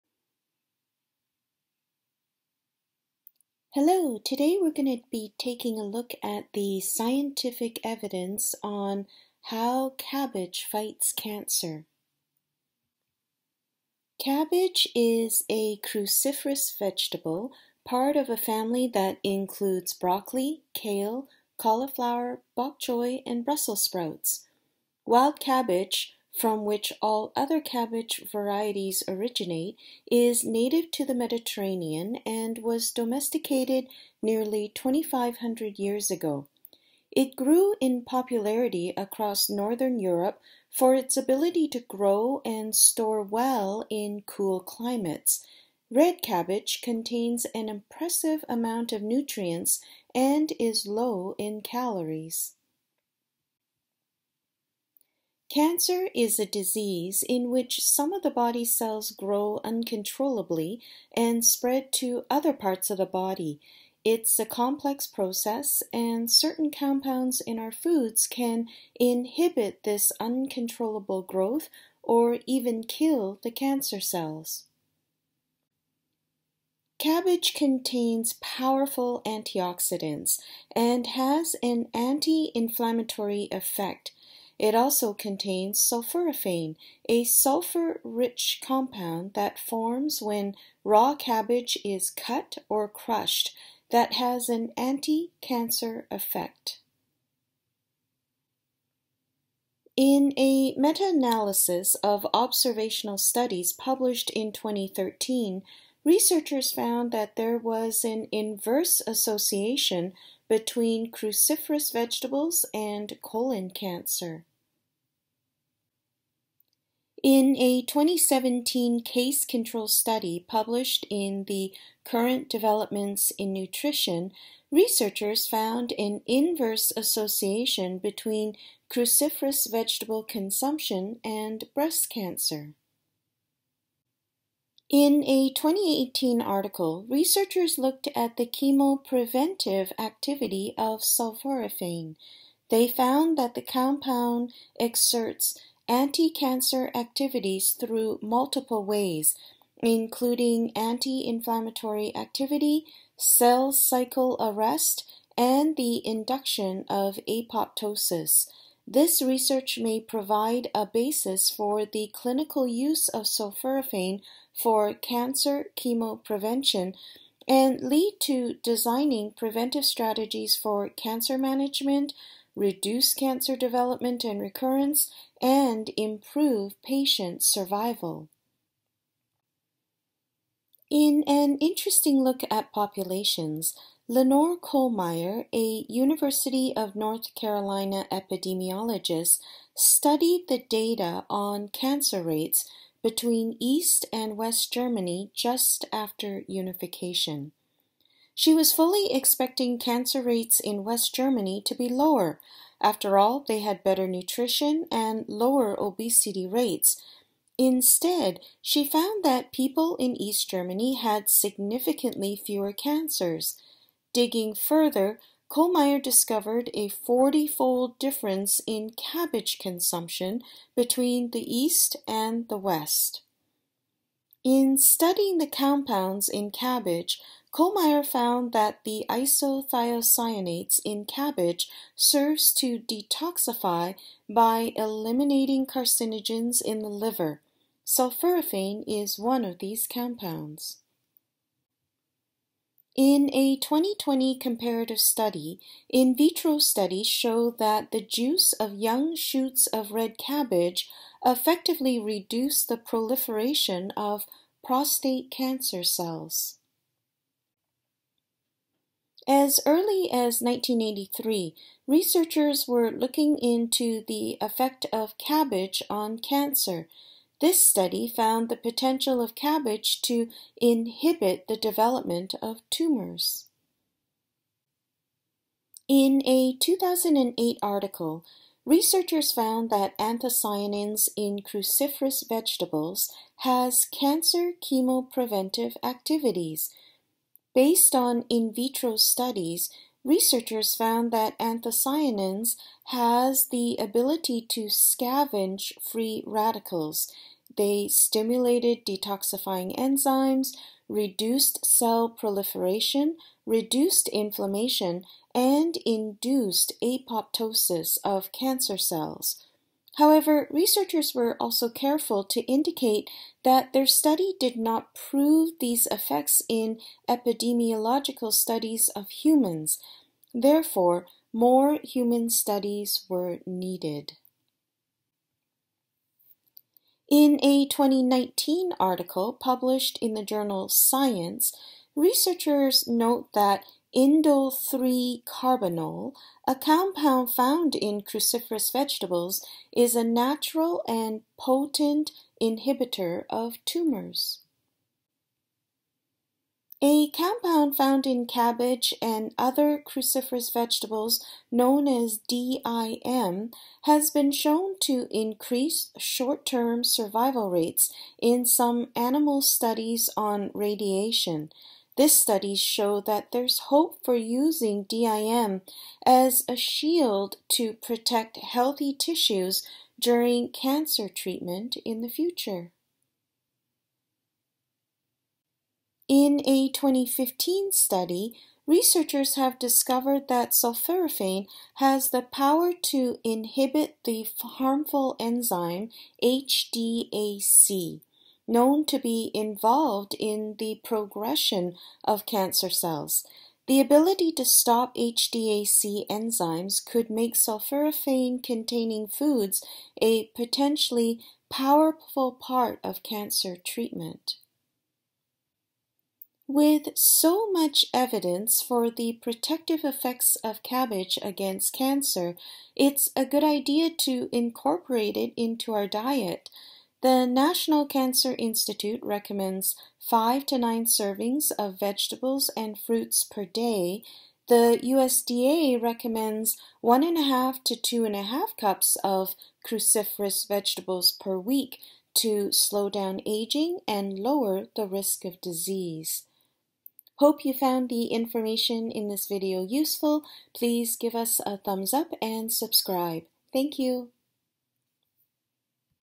How Cabbage Fights Cancer
Learn about the scientific evidence for how cabbage fights cancer.